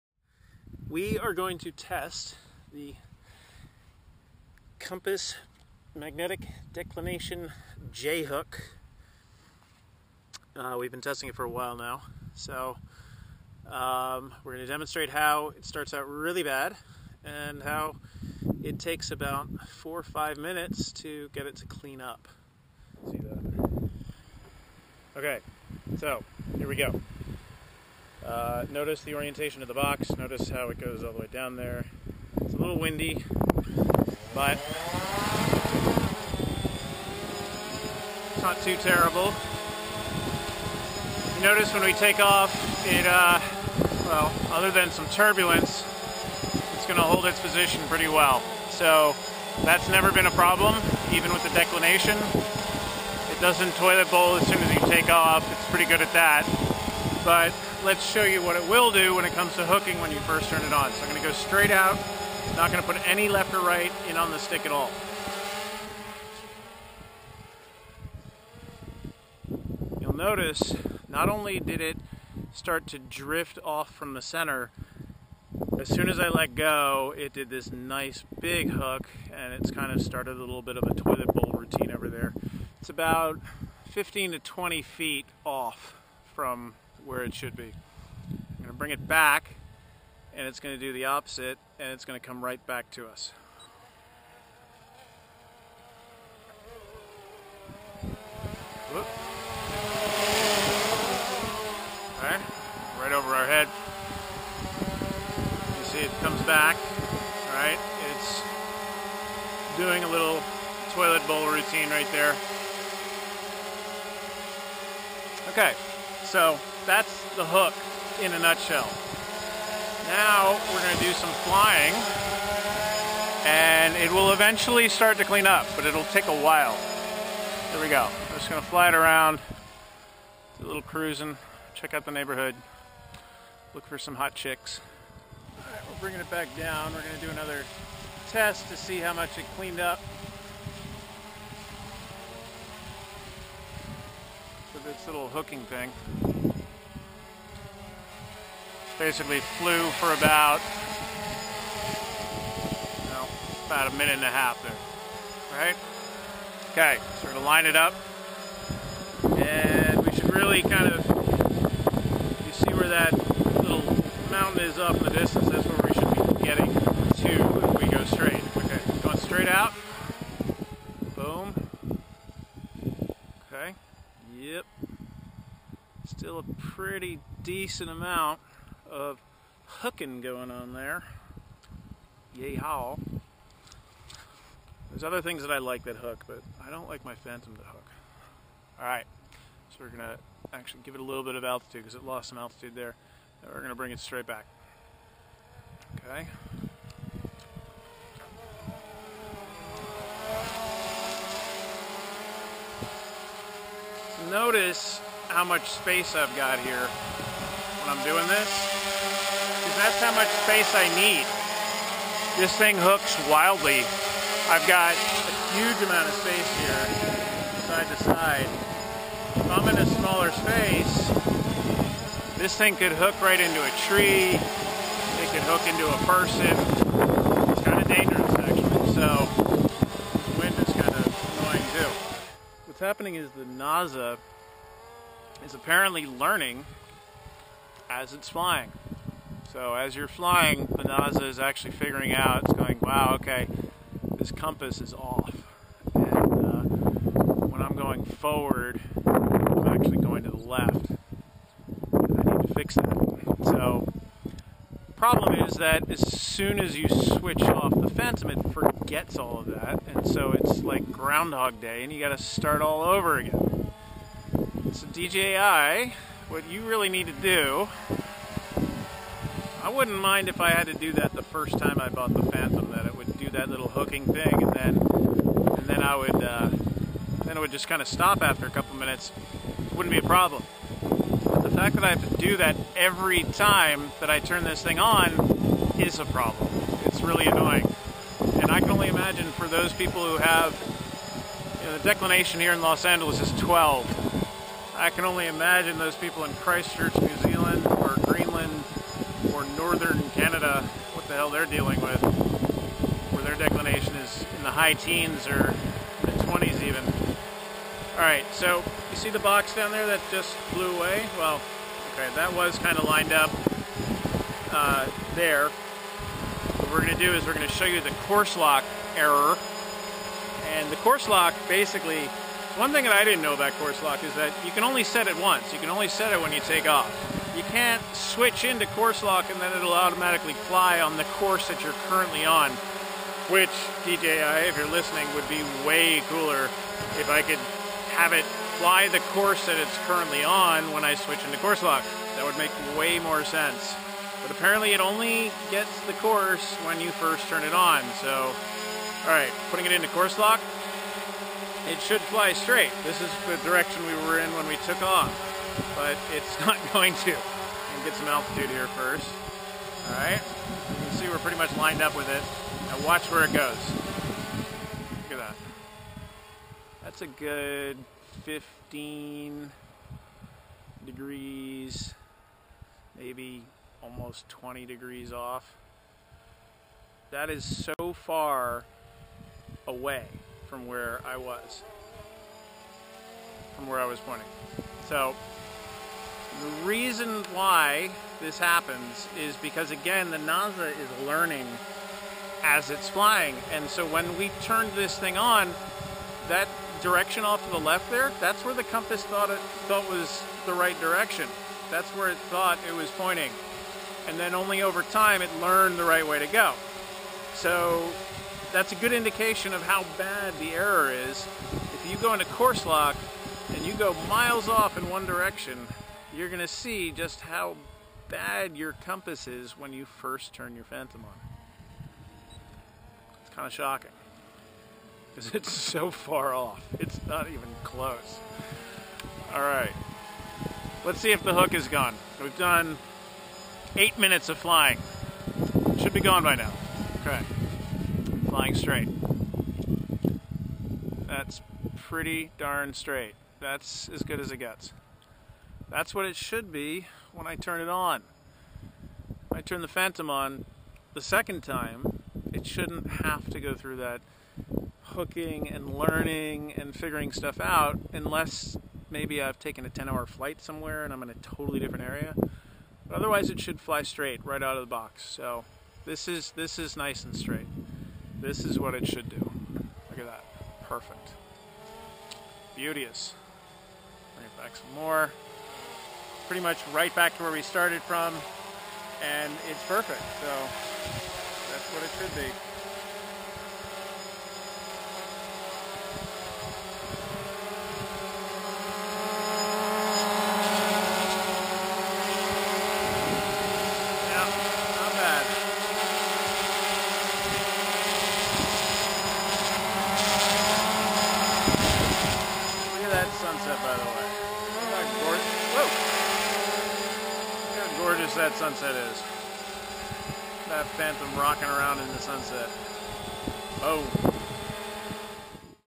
DJI's Compass Problem
In many parts of the world, the DJI Phantom 2 and 2 Vision won't fly straight when you take them out of the box. This is because the Naza flight controller doesn't do anything to adjust for magnetic declination. Instead, the Naza slowly adjusts to the declination over several minutes of flying. This wouldn't be so bad if the Naza remembered this adjustment on your next flight. But it does't and you have to start over every time. By the time the Phantom has figured out how to fly straight, you only have a few minutes of flying left.

Meanwhile, it is trivial to adjust automatically for declination. A few lines of code. Your GPS coordinates are used to find the local declination in a 1.6Kb lookup table and your compass reading is adjusted by that value. DJI, please fix this!

As of March 22, 2014, this impacts all Phantom 2 and P2V quad-rotors using the latest firmware (1.08). Multiple users have reported this problem in:

Western US and Canada, Northeastern US and Canada, Northeastern South America, Eastern Australia, New Zealand, Southern Africa.

If you have this problem with your P2 or P2Z, report it